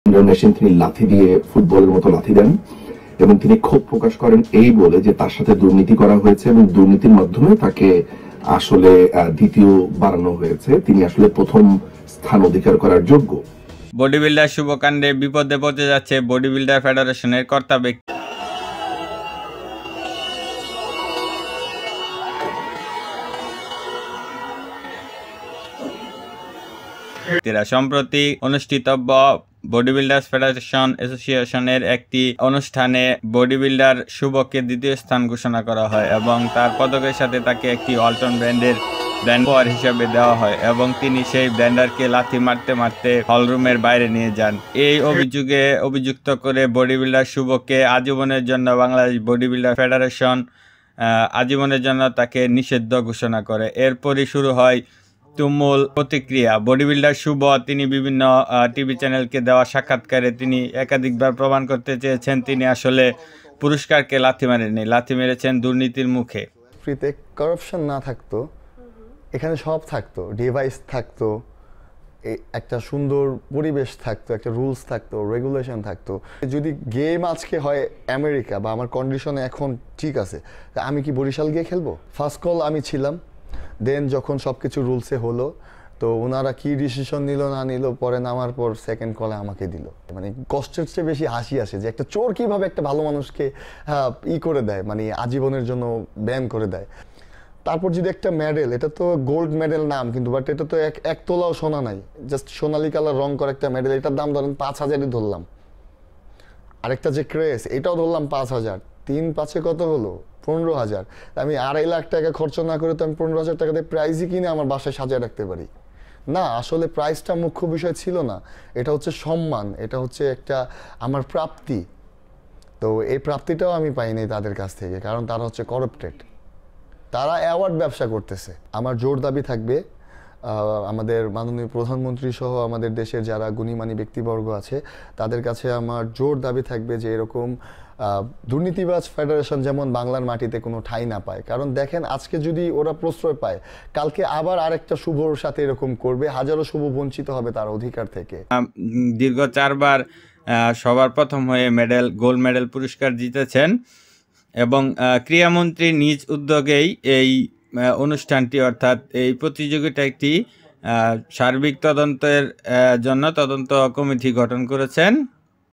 বডিবিল্ডার জাহিদ হাসার শুভ মাননীয় প্রধানমন্ত্রী শেখ হাসিনার কাছে বিচার দাবী করলেন ফেডারেশনে কর্তাদের
Bodybuilder Shuvo জাহিদ হাসান শুভ অশ্লীল ও অশোভনীয় আচরণ করেছেন শুভ, এমন অভিযোগ ফেডারেশনের। জাহিদ হাসান শুভর এই বিষয় নিয়ে তদন্ত কমিটি গঠন করেছে যুব ও ক্রীড়া মন্ত্রণালয়। জাহিদ হাসান শুভর এই বিষয় নিয়ে তদন্ত কমিটি গঠন করেছে যুব ও ক্রীড়া মন্ত্রণালয়।